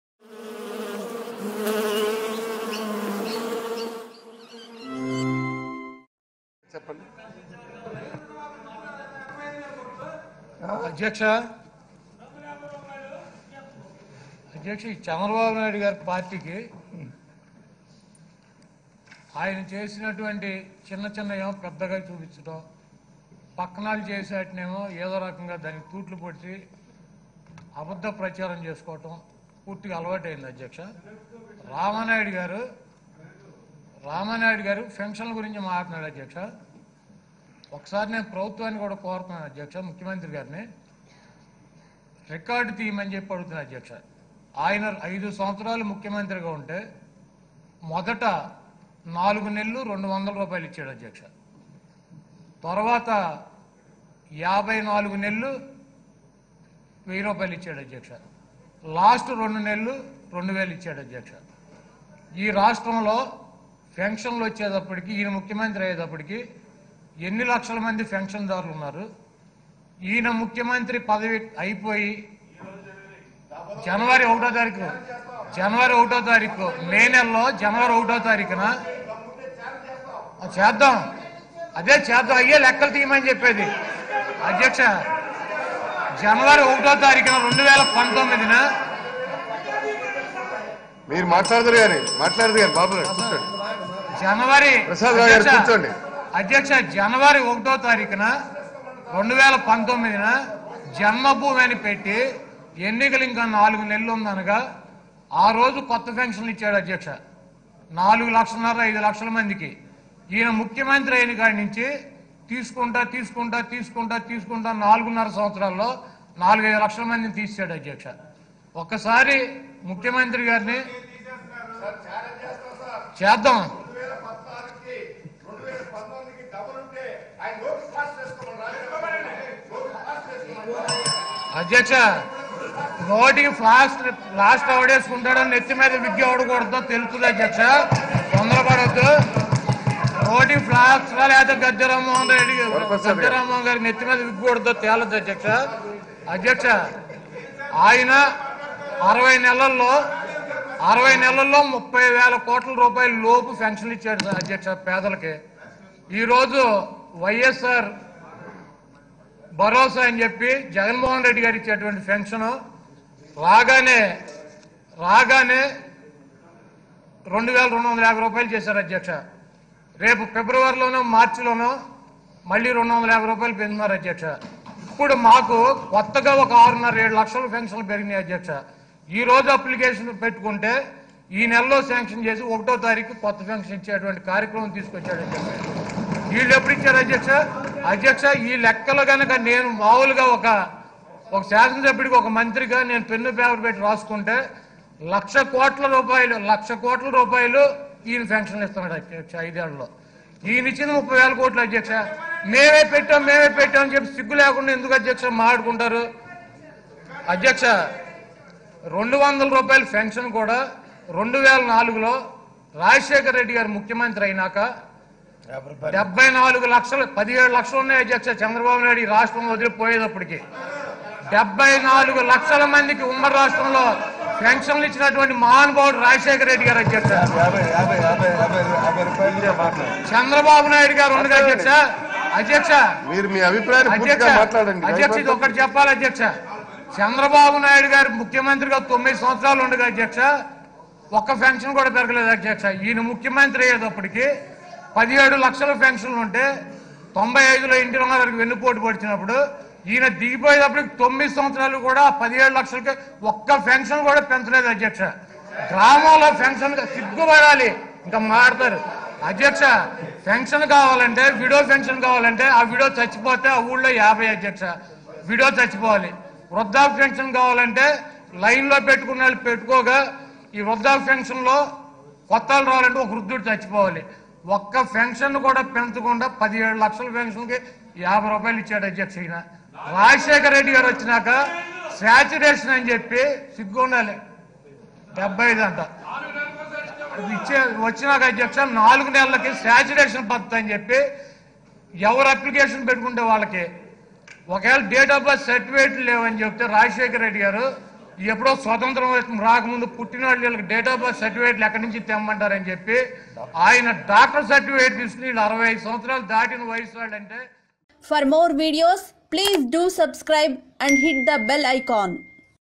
Kodali Nani satirical comments on TDP MLA | AP Assembly | Chandrababu | NewsSting
New Age Portal. Newspaper, TV & radio rolled into one. It's text, visual & audio. Brings insights, commentary & analyses to the reader/viewer/listener.